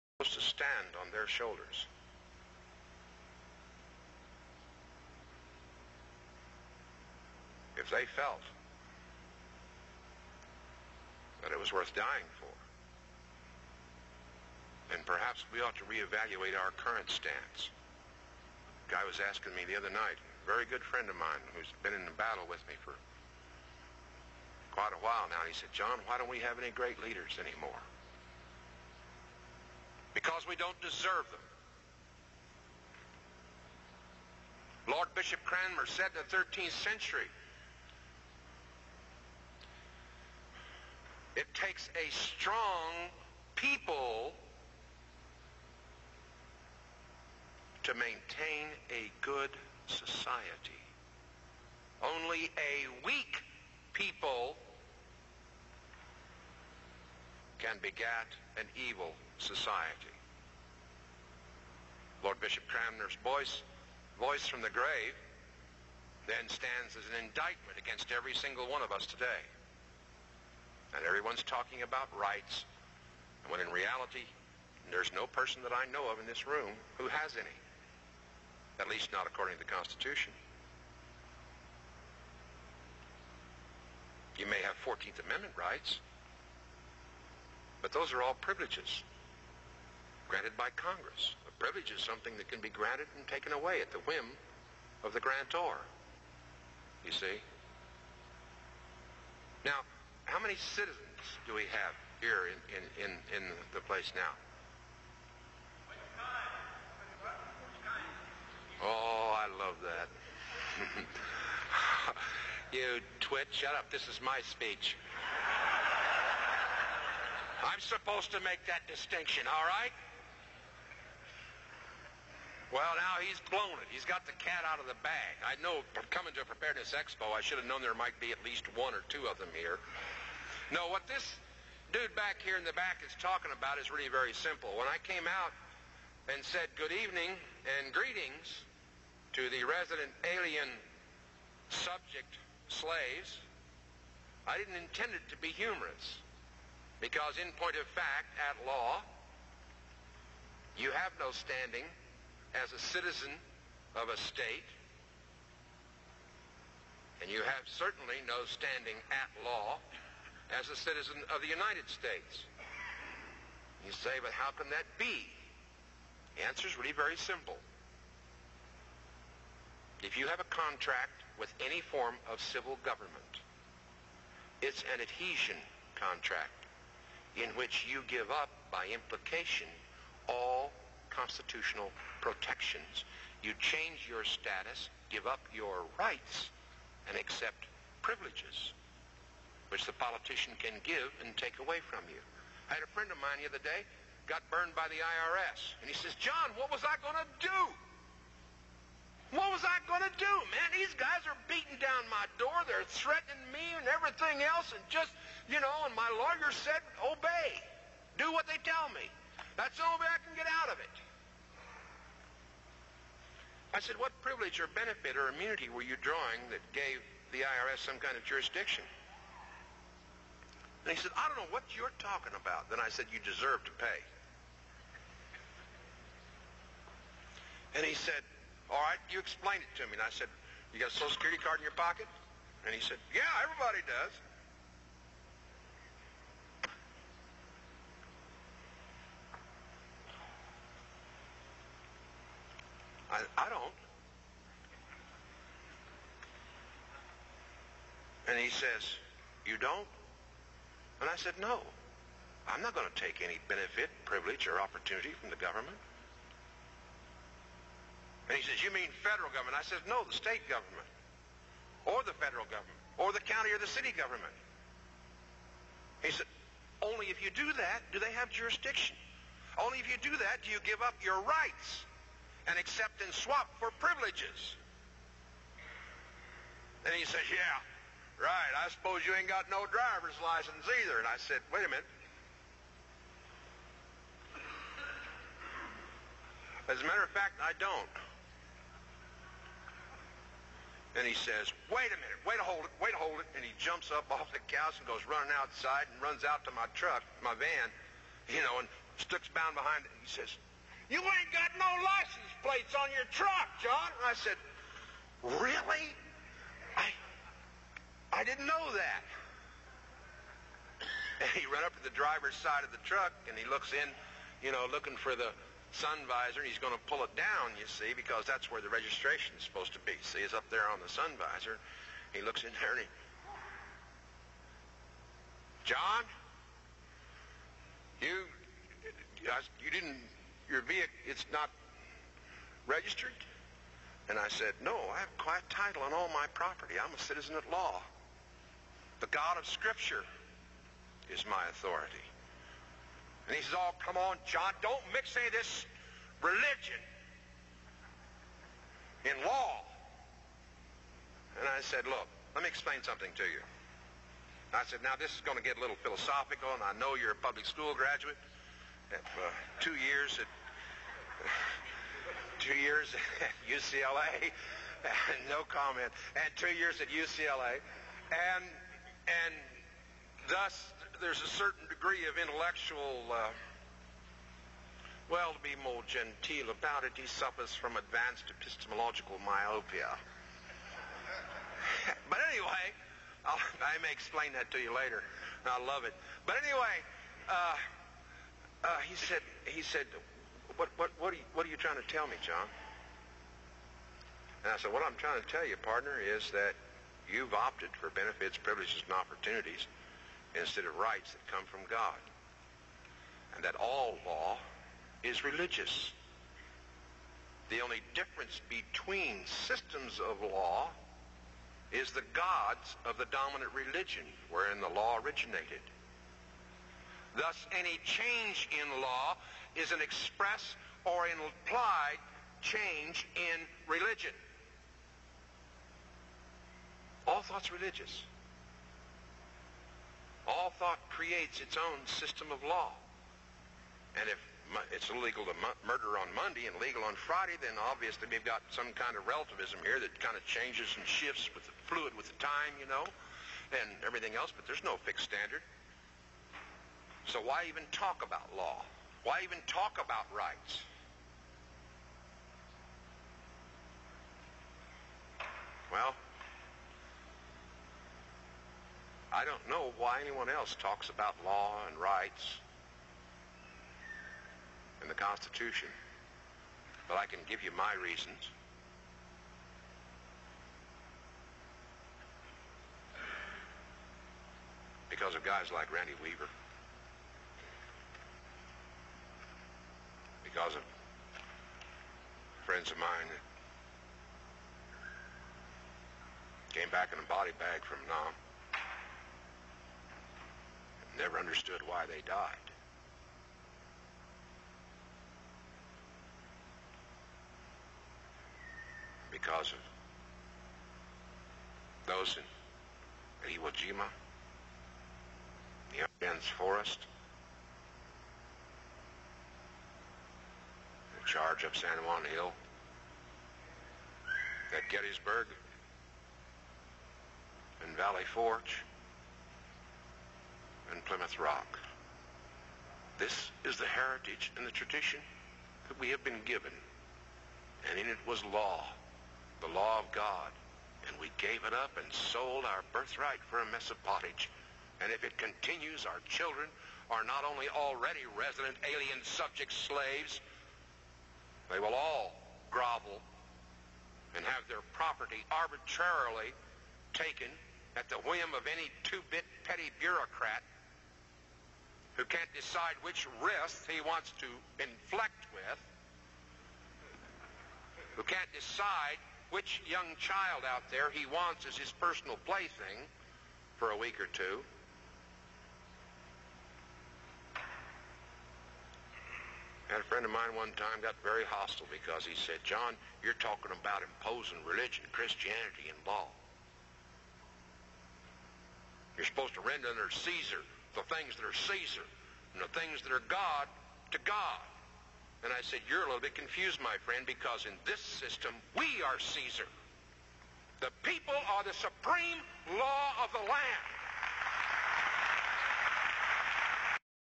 John Quade 2 All Law is Religious 144p
All Law is Religious
6:00 - All Law is Religious

JOHN QUADE SPEECH

transcript

INTRODUCTION: John Quade is a well-known actor. He appeared in more than 120 feature films, television shows, including Roots, many others which you may remember... The Sting. And John uses his talents to communicate to audiences in real-life dramas. Give a real hand of applause if you will to John Quade; so good to have him at Preparedness Expo.